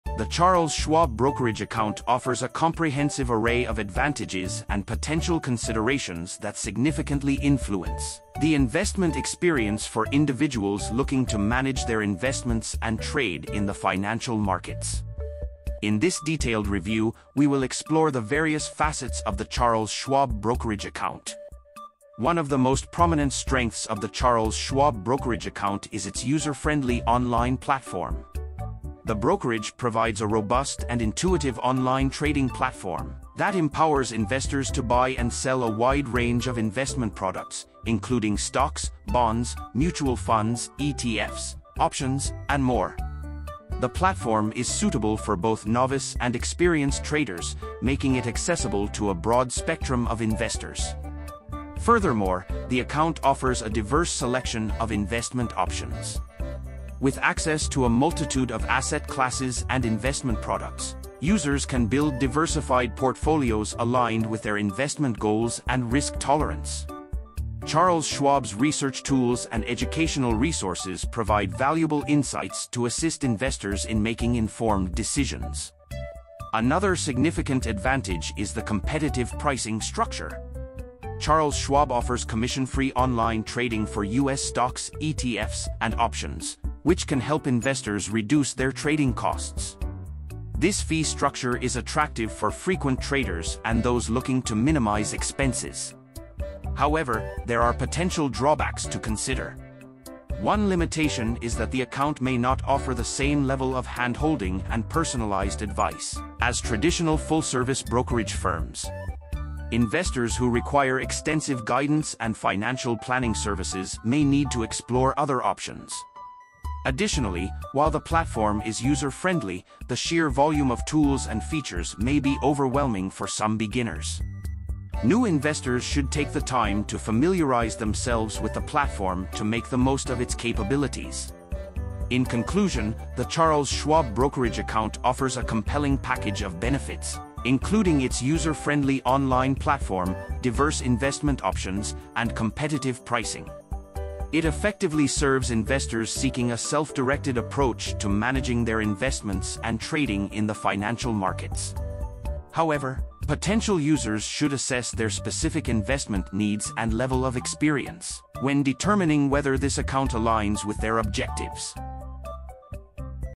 🔥 Charles Schwab Brokerage Account Review: Pros and Cons of Online Investing
The Charles Schwab Brokerage Account unveils a spectrum of advantages and potential considerations that significantly shape the investment experience for individuals seeking a flexible and comprehensive online platform for managing their investments. In this in-depth analysis, we will delve into the various facets of the Charles Schwab Brokerage Account.

One of the most notable strengths of the Charles Schwab Brokerage Account is its user-friendly online platform. The brokerage provides an intuitive and feature-rich online trading platform that caters to investors of all levels of experience. Whether you're a seasoned trader or a novice investor, the platform offers tools and resources to help you make informed investment decisions.

Furthermore, the account offers access to a wide range of investment options. From stocks and bonds to mutual funds, ETFs, options, and more, Charles Schwab's brokerage account allows investors to create diversified portfolios tailored to their specific investment goals and risk tolerance. The platform's research tools and educational materials further empower users to make well-informed choices.

Another significant advantage is the competitive pricing structure. Charles Schwab's commission-free online trading for U.S. stocks, ETFs, and options can help investors reduce their transaction costs, making it an attractive option for those who actively trade or seek to minimize expenses.

However, there are potential drawbacks to consider. One limitation is that the Charles Schwab Brokerage Account may not provide the same level of personalized advice and financial planning services as traditional full-service brokerage firms. Investors who require extensive guidance and hands-on assistance may find these services elsewhere.

Additionally, while the platform is user-friendly, its extensive range of tools and features can be overwhelming for beginners. New investors should take the time to familiarize themselves with the platform to maximize its capabilities fully.

In conclusion, the Charles Schwab Brokerage Account offers a compelling package of benefits, including its user-friendly online platform, diverse investment options, and competitive pricing. It effectively serves investors seeking a self-directed approach to managing their investments and trading online. However, potential users should carefully assess their specific investment needs and level of experience to determine whether this account aligns with their financial objectives.